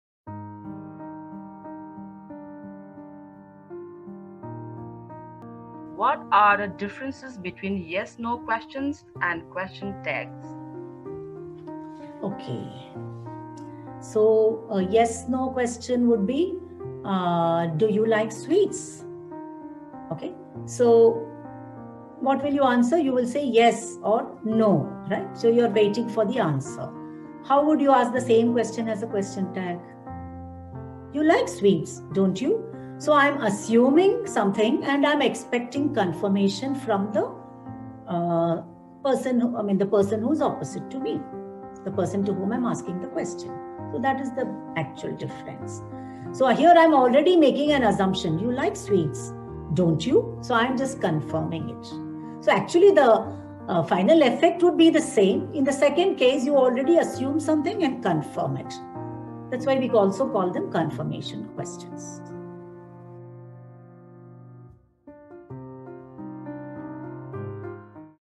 How to Ask Questions? Yes/No Questions vs Question Tags | UrbanPro
Many ESL learners quickly show improvement and become adept at making sentences in response to questions asked by the trainer during the classes. However, when they are required to frame questions, they find it challenging to come up with clear and grammatically accurate questions. This activity-based session will guide you through the types of questions in English, their structures, and examples in a highly interactive manner.